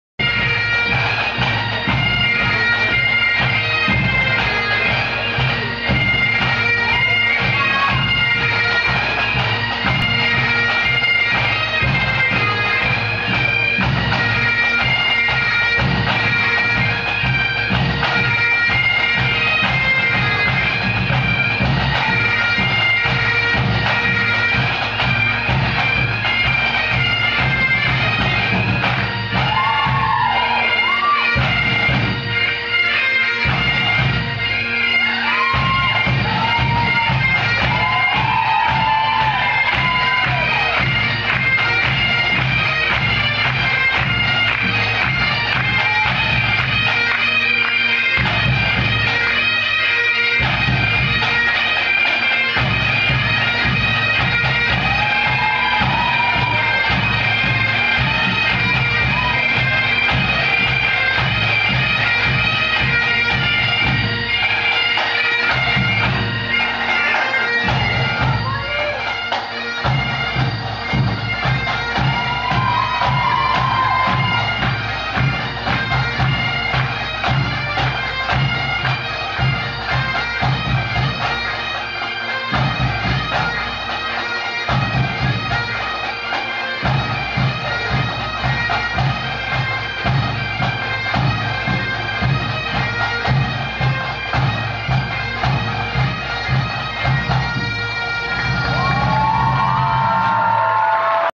2011 Thistle Dance - Show 2, part 1
Highland Thistle Pipe Band, part 1 of 2nd show during Thistle Dance 2011